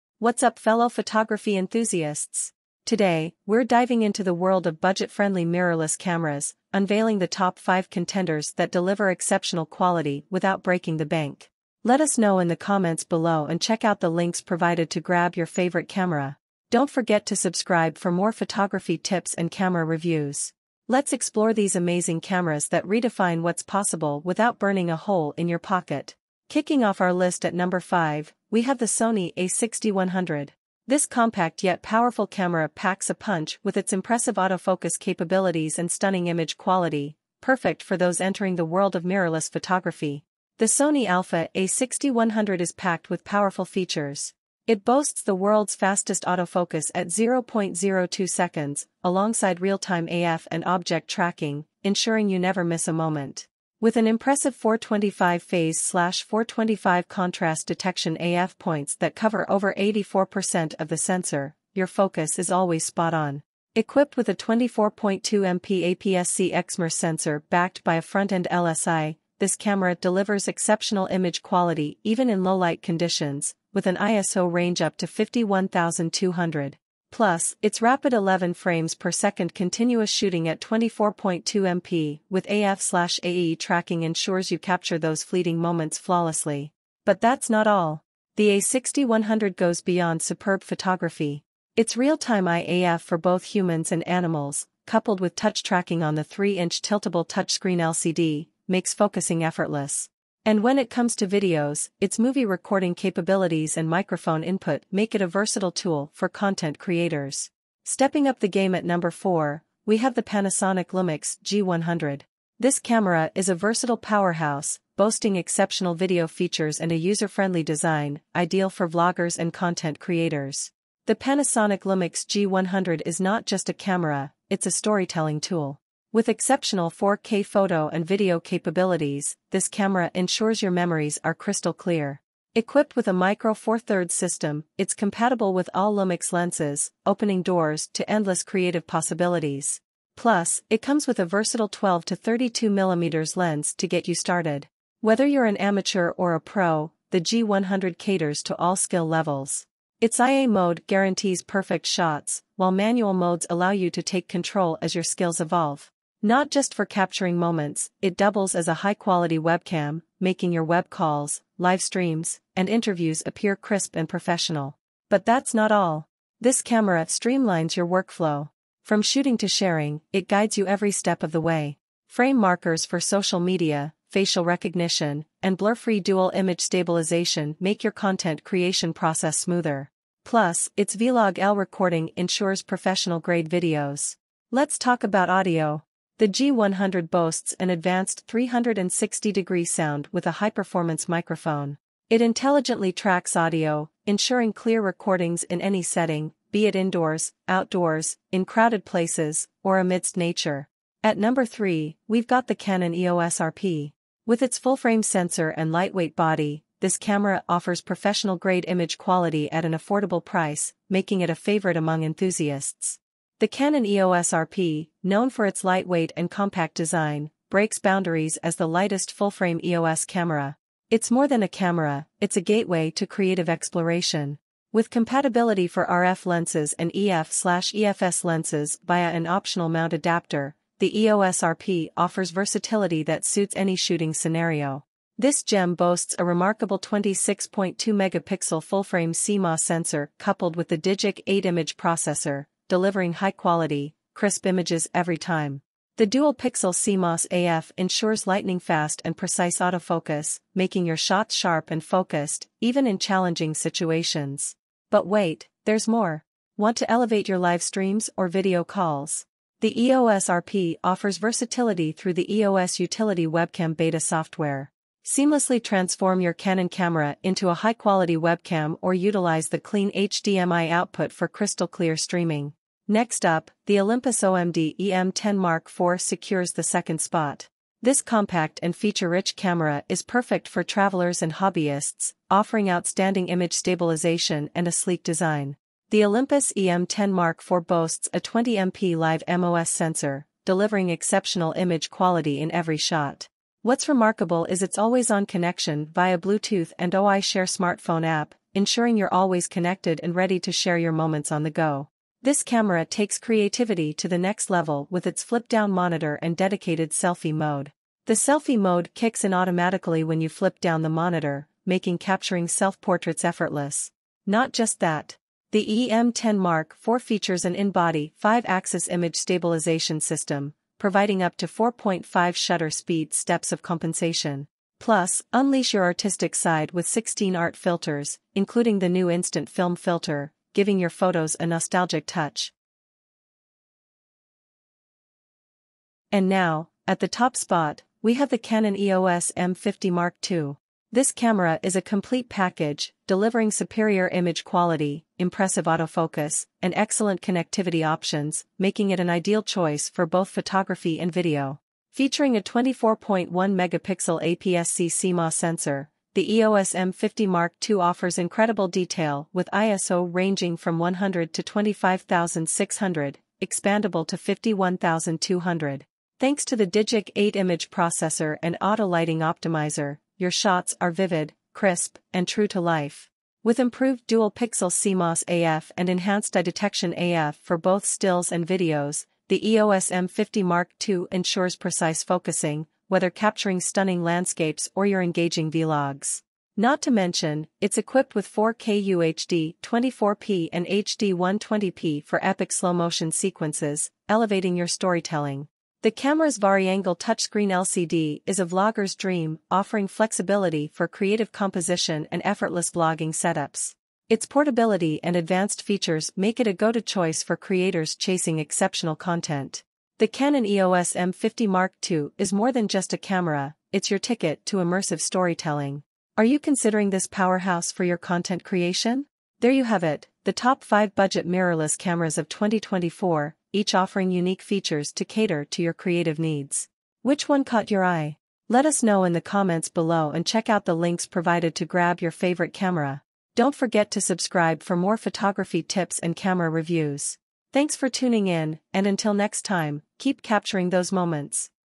Top 5 Best Budget Mirrorless Cameras of 2024 - Unleashing Quality on a Budget!
"Discover the top 5 budget-friendly mirrorless cameras of 2024 that deliver exceptional quality without breaking the bank! Join us as we explore these remarkable cameras, perfect for photography enthusiasts seeking outstanding performance at an affordable price. From the Sony A6100 to the Canon EOS M50 Mark II, each camera on this list redefines what's possible without compromising on quality. Dive into our comprehensive review and find the perfect mirrorless camera that fits your budget and creativity!"

►UK Links◄
➜ 5. Sony A6100
➜ 4. Panasonic Lumix G100
➜ 3. Canon EOS RP
➜ 2. Olympus OM-D E-M10 Mark IV
➜ 1. Canon EOS M50 Mark II

We have just laid out the top 5 best Budget Mirrorless Cameras 2024. In 5th place is the Sony A6100, our pick for the best value budget mirrorless camera. In 4th place is the Panasonic Lumix G100, our pick for the best vlogging budget mirrorless camera. In 3rd place is the Canon EOS RP, our pick for the best full frame budget mirrorless camera. In 2nd place is the Olympus OM-D E-M10 Mark IV, our pick for the best runner-up budget mirrorless camera. In 1st place is the Canon EOS M50 Mark II, our pick for the best overall budget mirrorless camera.